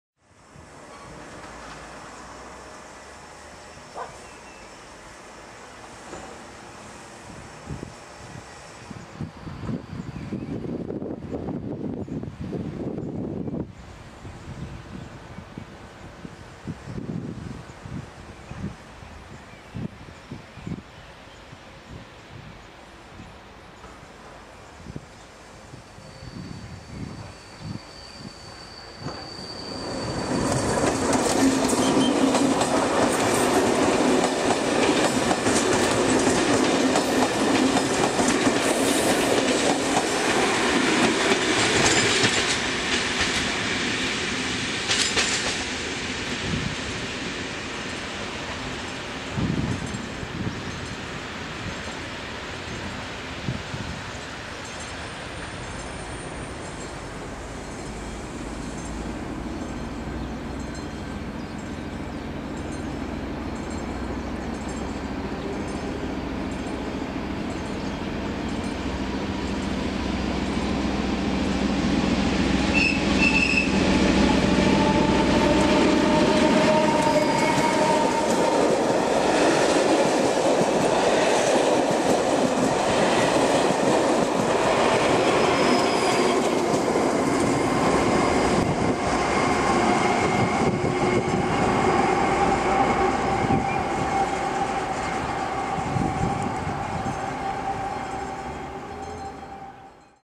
754.049+051 mimořádně na R 1144 | 13.06.2018
Dne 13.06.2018 se po menší přeháňce na rychlíku 1144 mimořádně ukázal bleskový brejlovec 754.049, kterému na postrku pomáhal další brejlovec 754.051. Děkuji fírovi za hezký pozdrav.
Na R 1147 jela retro hydra 854.021.